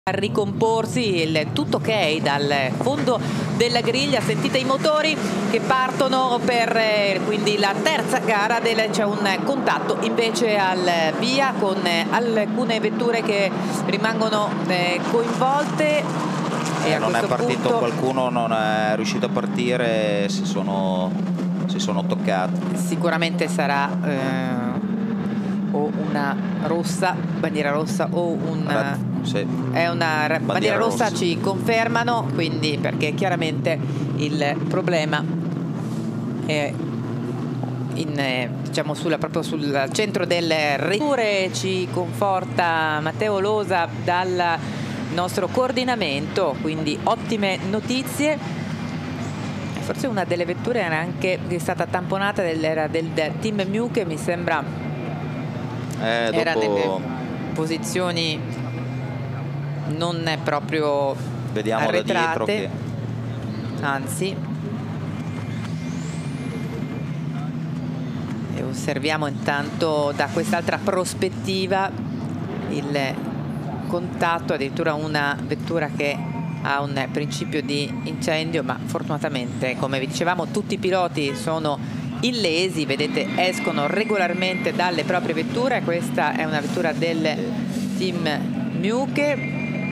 Italian Formula 4 2016. Misano. Pile up at start
Red flagged Formula 4 crash at Misano

Webmoney:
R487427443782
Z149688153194